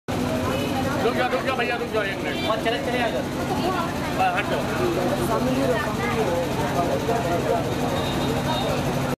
काजी कटरा की दुर्गा पूजा समिति ने कराया 11 कन्याओं का सामूहिक विवाह
बहराइच: शारदीय नवरात्र पर जहां एक ओर देवी भक्ति की धूम रही, वहीं सामाजिक बुराइयों के खिलाफ भी अभियान चलाया गया. इस अभियान के तहत काजी कटरा दुर्गा पूजा समिति की ओर से नवमी के दिन 11 कन्याओं का सामूहिक विवाह कराया गया.